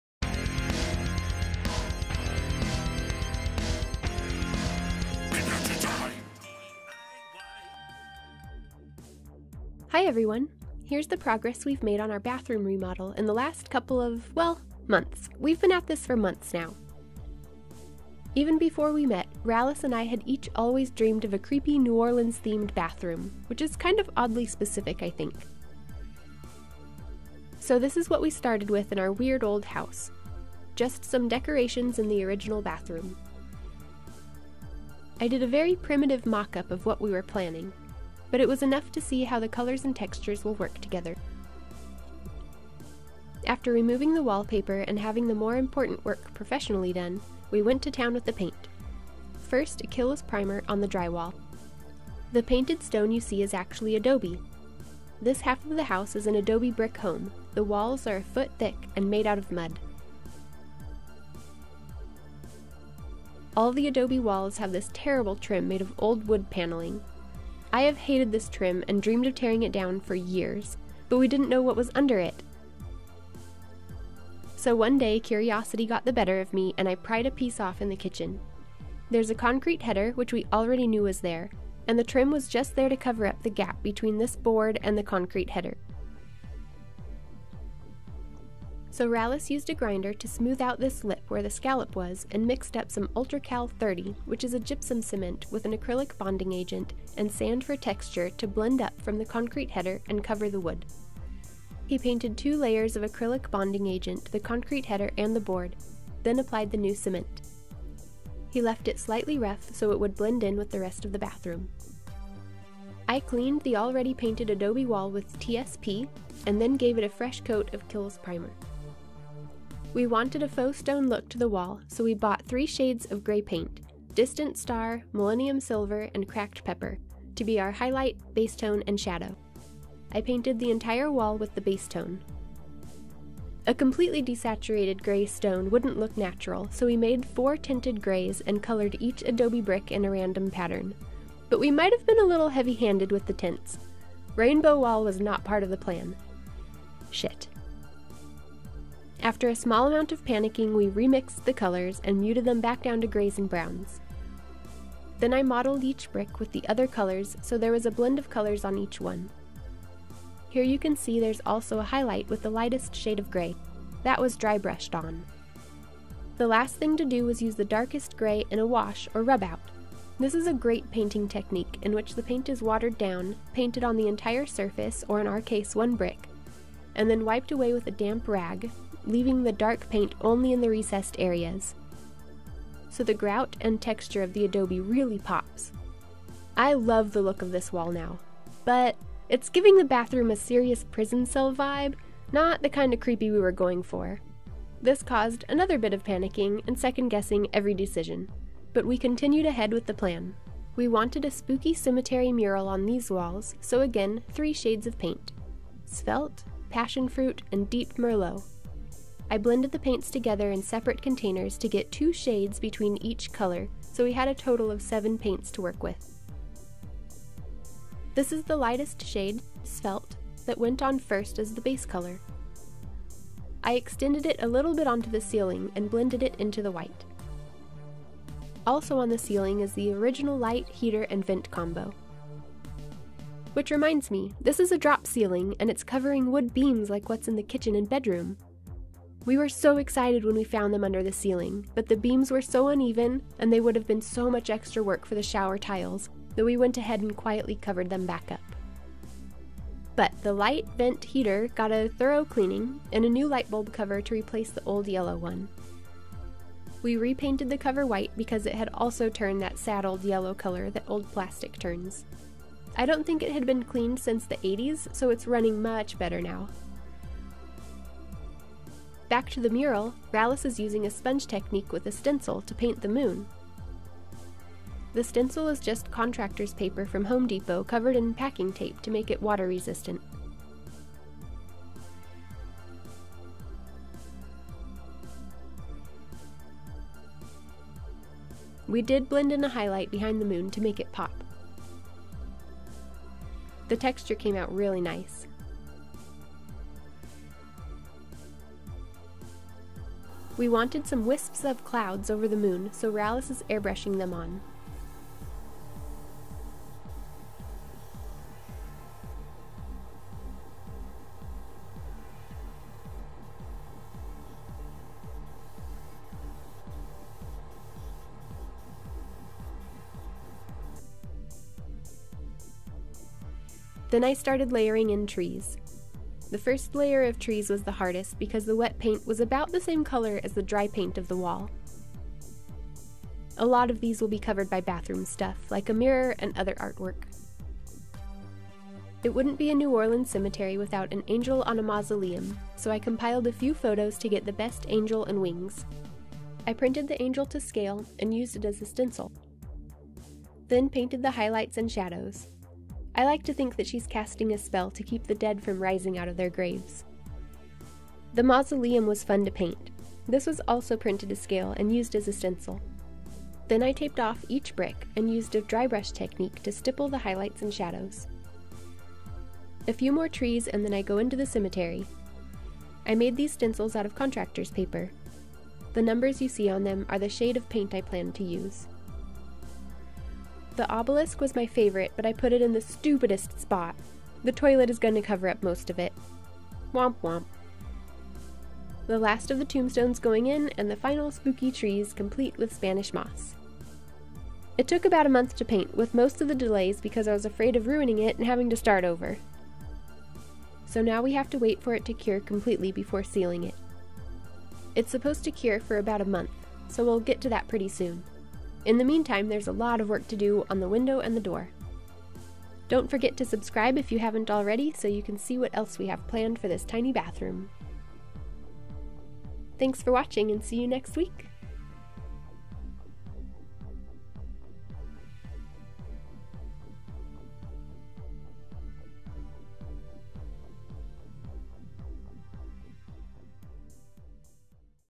Painting Faux Stone and a Cemetery Mural
Something new for you - a peak at the progress of our creepy bathroom remodel! This is the master bath in an adobe home that was built in 1947. It's tiny! The tile work was professionally done. This video just goes over how we painted the spooky cemetery mural and painted the adobe bricks to look like stone.